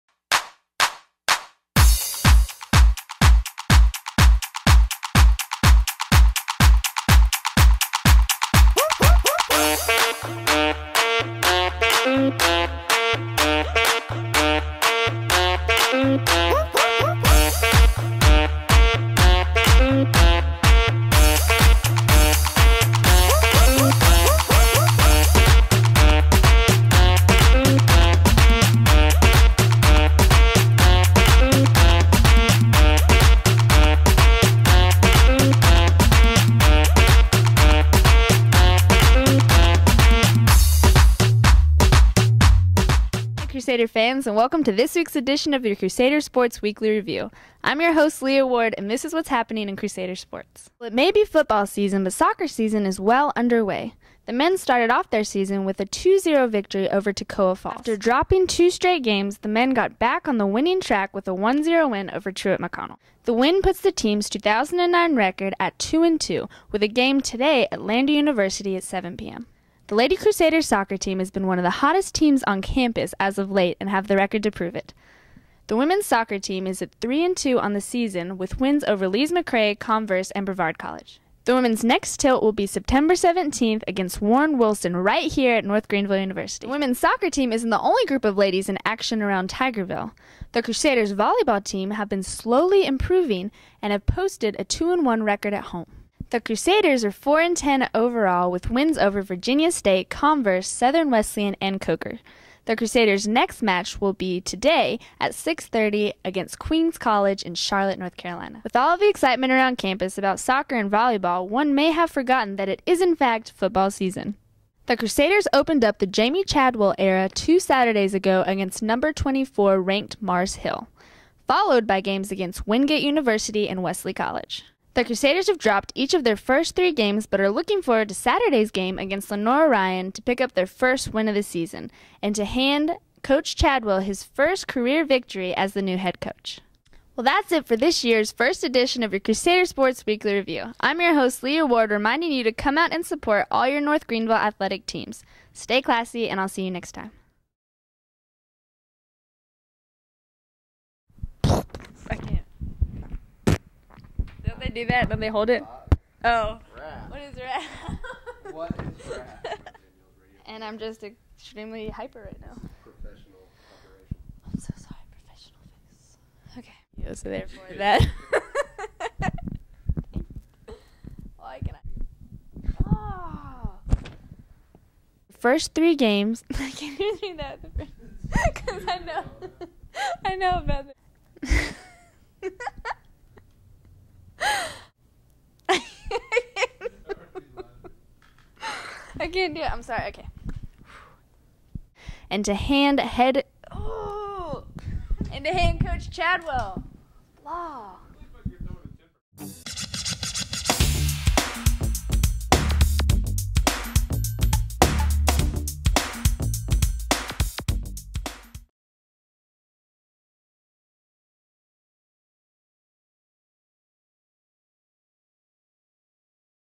CRUSADER SPORTS WEEKLY REVIEW
Find out what is going on in Crusader athletics including, Scores and dates of future games.